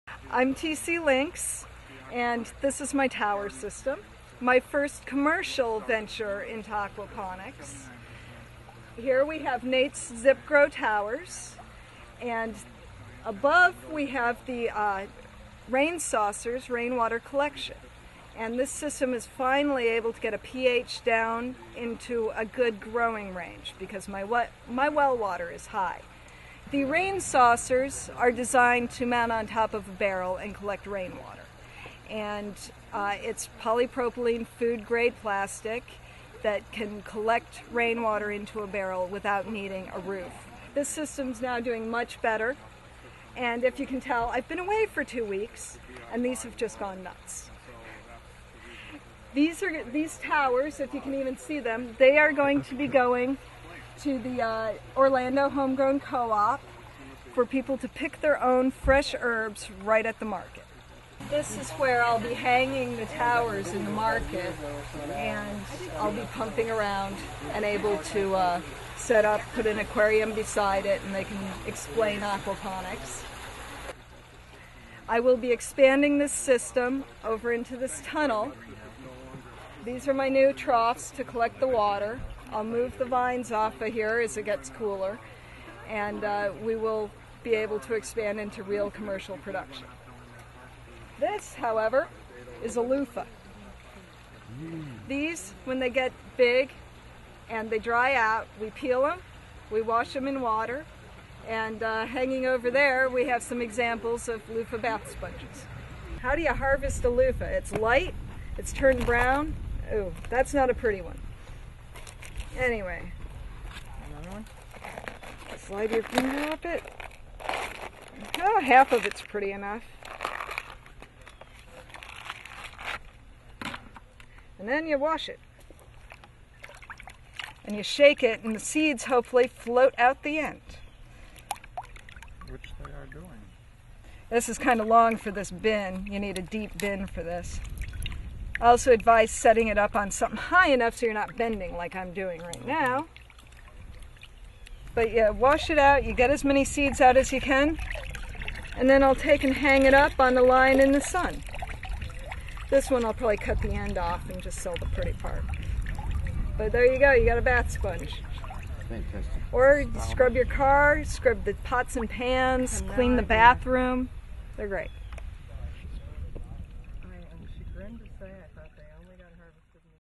Aquaponics Basil Grow Towers
Growing Basil Towers in an aquaponics system with Aleece Landis or TCLynx.